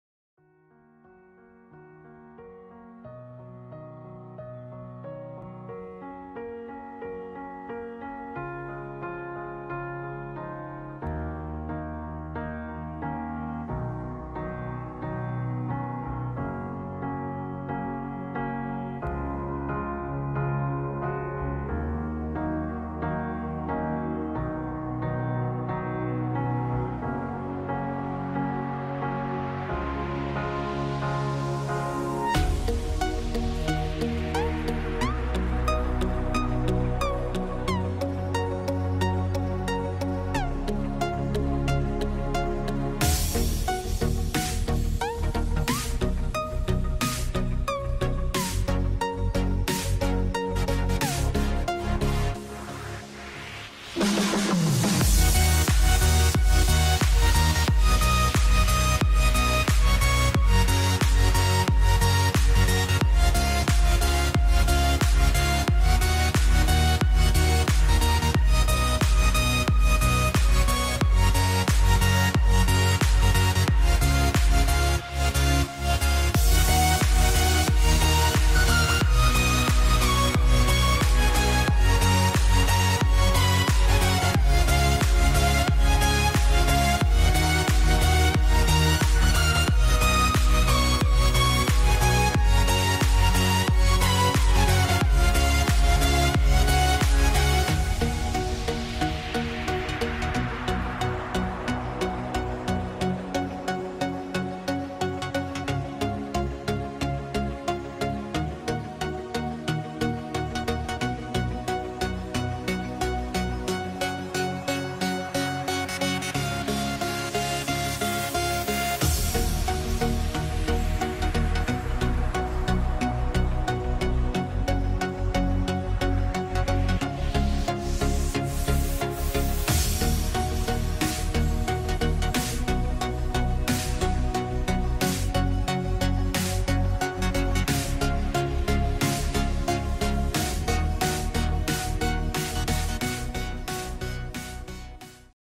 Digital Painting Time Lapse - Blue Iris
Another speedart video of a digital painting I made recently.